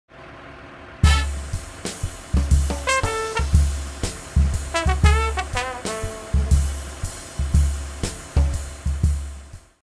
Mountain Crane
A familiar sight in downtown Boise, winter 2016.  This crane is set up on Main street, the sight of a new transportation hub.